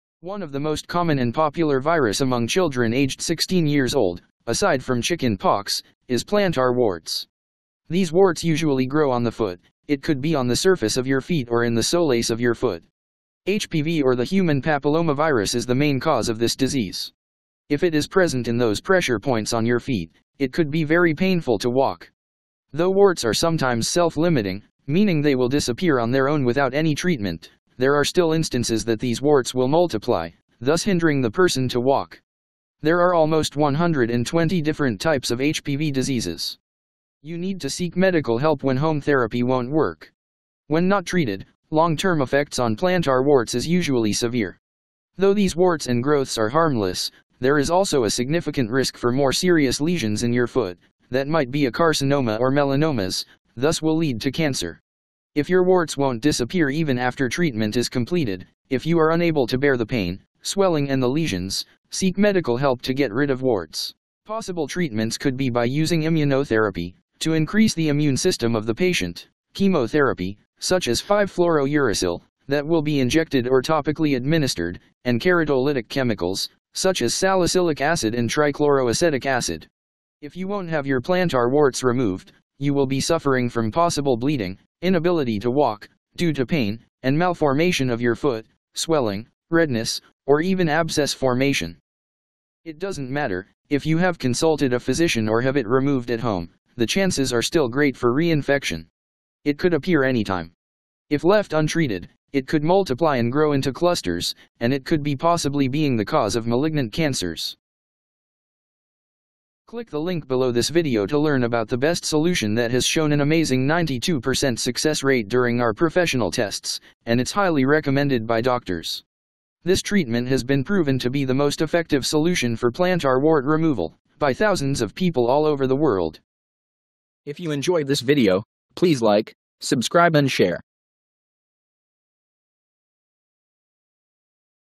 Plantar Wart Removal WARNING! Shocking Truth at The End
- The best plantar wart removal treatment that has shown an amazing 92% success rate during our professional tests and it is highly recommended by doctors.

One of the most common and popular virus among children aged 16 years old aside from chicken pox is plantar warts. These warts usually grow on the foot; it could be on the surface of your feet or in the soles of your foot. HPV or the Human Papillomavirus is the main cause of this disease. If it is present in those pressure points on your feet, it could be very painful to walk. Though warts are sometimes self-limiting, meaning they will disappear on their own without any treatment. There are still instances that these warts will multiply thus hindering the person to walk. There are almost 120 different types of HPV diseases.

You need to seek medical help when home therapy won’t work. When not treated, long-term effects on plantar warts is usually severe. Though these warts and growths are harmless there is also a significant risk for more serious lesions in your foot that might be a carcinoma or melanomas thus will lead to cancer. Seek medical help if you want these warts to be removed, if you are unable to bear the pain, swelling and the lesions, if you developed a fever, re-infected after treatment and it won’t disappear even after treatment is completed.

How to get rid of plantar warts

Possible treatments could be by using immunotherapy to increase the immune system of the patient, chemotherapy such as 5-fluoro-uracil that will be injected or topically administered and keratolytic chemicals such as salicylic acid and trichloroacetic acid.

If you won’t have your plantar warts removed, chances are you will be suffering from possible bleeding, inability to walk due to pain and malformation of your foot, swelling, redness, or even abscess formation.

It doesn’t matter if you have consulted a physician or have it removed at home, the chances are still great for re-infection. It could appear anytime. If left untreated it could multiply and grow into clusters and it could be possibly being the cause of malignant cancers.